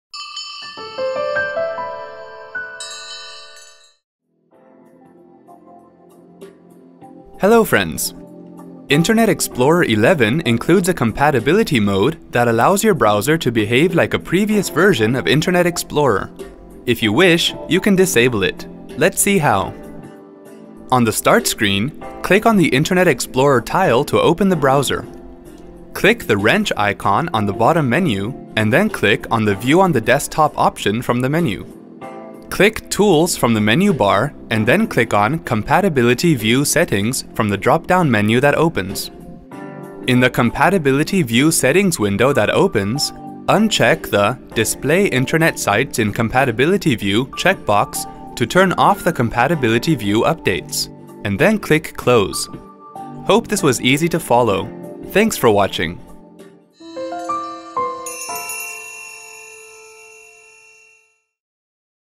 How to turn off Compatibility View Updates in Internet Explorer® 11 on a Windows® 8.1 PC
Want to turn off Compatibility View Updates in Internet Explorer® 11 on a Windows® 8.1-based PC? Internet Explorer® 11 includes a Compatibility Mode that allows your browser to behave like a previous version of Internet Explorer®. Follow the easy steps shown in this video.

The tech support team at iYogi™ can be accessed by dialing toll-free number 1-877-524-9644 from the U.S. or Canada, or by dialing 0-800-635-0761 from the U.K.